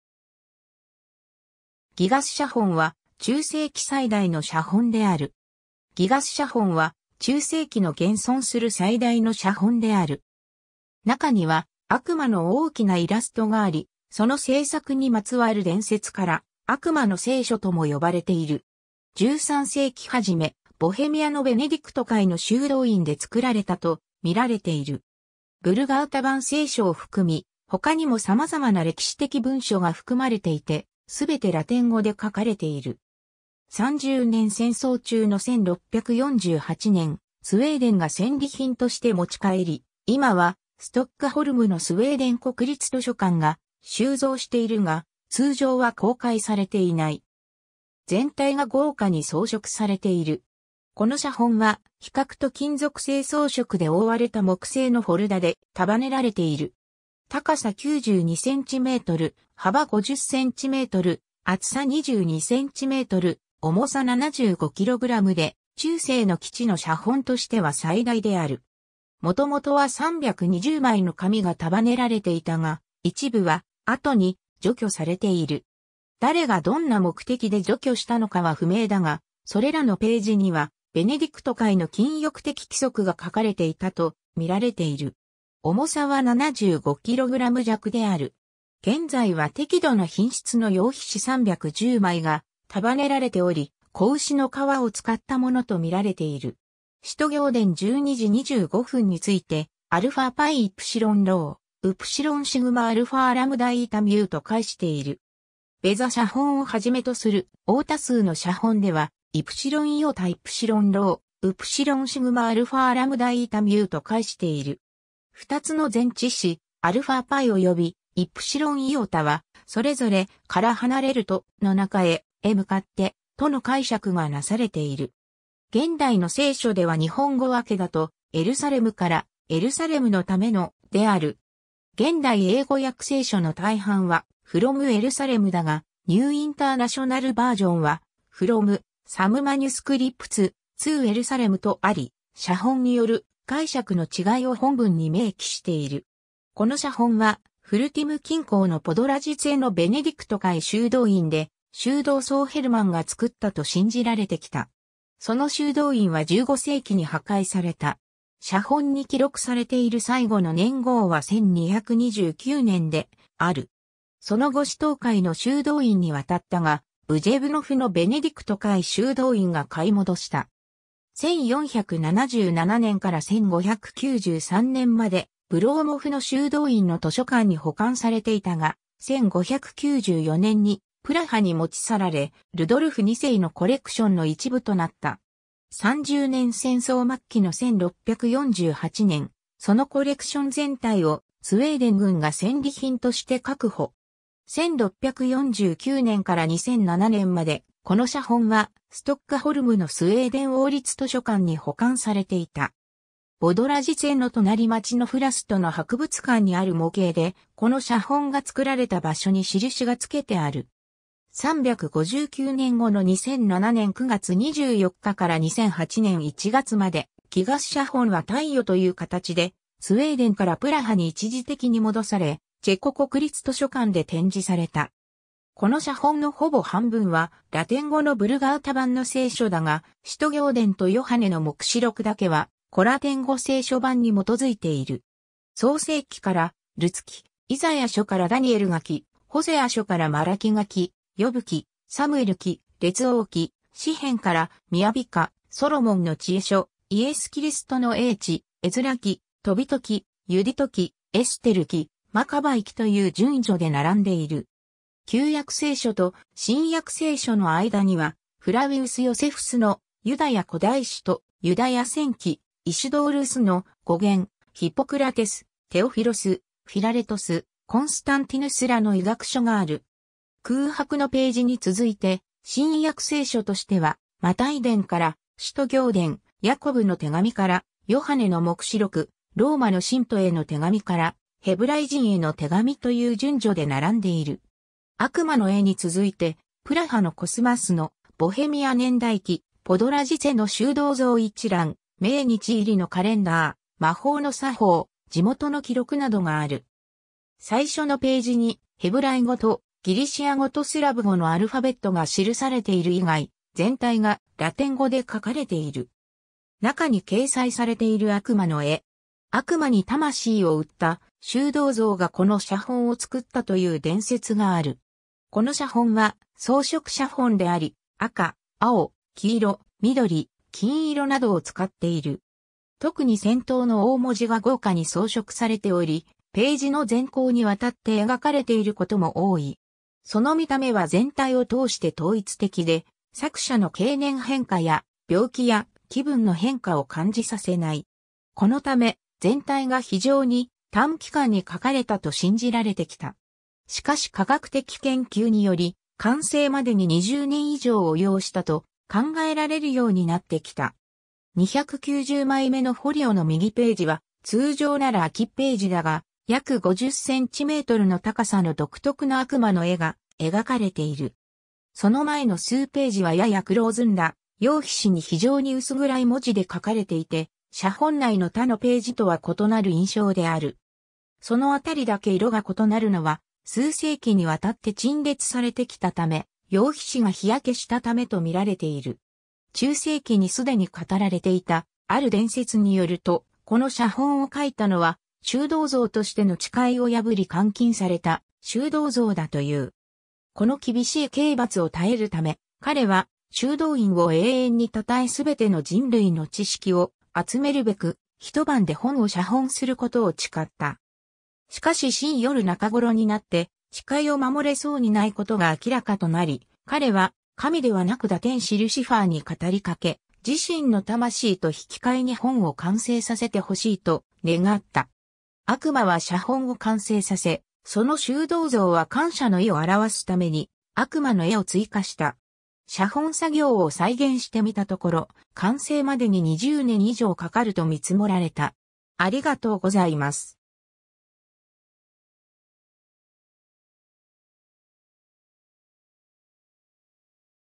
ギガス写本
ギガス写本は中世期最大の写本である。
 ギガス写本（ギガスしゃほん、Codex Gigas）は、中世期の現存する最大の写本である。
中には悪魔の大きなイラストがあり、その製作にまつわる伝説から悪魔の聖書 (Devil's Bible) とも呼ばれている。
13世紀初め、ボヘミア（現在のチェコ）のベネディクト会の修道院で作られたと見られている。
ヴルガータ版聖書を含み、他にも様々な歴史的文書が含まれていて、全てラテン語で書かれている。
三十年戦争中の1648年、スウェーデンが戦利品として持ち帰り、今はストックホルムのスウェーデン国立図書館が収蔵しているが、通常は公開されていない。
 全体が豪華に装飾されている。
 この写本は皮革と金属製装飾で覆われた木製のフォルダで束ねられている。
高さ92cm、幅50cm、厚さ22cm、重さ75kgで、中世の既知の写本としては最大である（そのため "gigas" すなわち「巨大」と呼ばれている）。
元々は320枚の紙が束ねられていたが、一部は後に除去されている。
誰がどんな目的で除去したのかは不明だが、それらのページにはベネディクト会の禁欲的規則が書かれていたと見られている。
重さは75kg弱である。
現在は適度な品質の羊皮紙310枚が束ねられており、子牛の皮を使ったものと見られている。
 使徒行伝 12:25についてαπο Ιερουσαλημ (from Jerusalem) と解している。
ベザ写本を始めとする大多数の写本ではεις Ιερουσαλημ (to Jerusalem) と解している。
二つの前置詞"απο"および"εις"はそれぞれ「～から離れる」と「～の中へ、～へ向かって」との解釈がなされている。
現代の聖書では日本語訳だと「エルサレムから」（口語訳聖書、新改訳聖書）、「エルサレムのための（任務を果たし）」（新共同訳聖書）である。
現代英語訳聖書の大半は"from Jerusalem"だがNew International Versionは"from {Some manuscripts 「to」} Jerusalem"とあり、写本による解釈の違いを本文に明記している。
 この写本は、フルティム近郊のポドラジツェのベネディクト会修道院で、修道僧ヘルマンが作ったと信じられてきた。
その修道院は15世紀に破壊された。
写本に記録されている最後の年号は1229年である。
その後シトー会の修道院に渡ったが、ブジェヴノフのベネディクト会修道院が買い戻した。
1477年から1593年までブロウモフの修道院の図書館に保管されていたが、1594年にプラハに持ち去られ、ルドルフ2世のコレクションの一部となった。
 三十年戦争末期の1648年、そのコレクション全体をスウェーデン軍が戦利品として確保。
1649年から2007年までこの写本はストックホルムのスウェーデン王立図書館に保管されていた。
ボドラジツェの隣町のフラストの博物館にある模型で、この写本が作られた場所に印がつけてある。
 359年後の2007年9月24日から2008年1月まで、ギガス写本は貸与という形でスウェーデンからプラハに一時的に戻され、チェコ国立図書館で展示された。
 この写本のほぼ半分はラテン語のヴルガータ版の聖書だが、使徒行伝とヨハネの黙示録だけは古ラテン語聖書版に基づいている。
創世記からルツ記、イザヤ書からダニエル書、ホセア書からマラキ書、ヨブ記、サムエル記、列王記、詩篇から雅歌、ソロモンの知恵書、イエス・キリストの叡智、エズラ記、トビト記、ユディト記、エステル記、マカバイ記という順序で並んでいる。
旧約聖書と新約聖書の間には、フラウィウス・ヨセフスの『ユダヤ古代誌』と『ユダヤ戦記』、イシドールスの『語源』、ヒポクラテス、テオフィロス、フィラレトス、コンスタンティヌスらの医学書がある。
空白のページに続いて、新約聖書としてはマタイ伝から使徒行伝、ヤコブの手紙からヨハネの黙示録、ローマの信徒への手紙からヘブライ人への手紙という順序で並んでいる。
悪魔の絵に続いて、プラハのコスマスの『ボヘミア年代記』、ポドラジツェの修道僧一覧、命日入りのカレンダー、魔法の作法、地元の記録などがある。
最初のページにヘブライ語とギリシア語とスラブ語のアルファベットが記されている以外、全体がラテン語で書かれている。
 中に掲載されている悪魔の絵。
悪魔に魂を売った...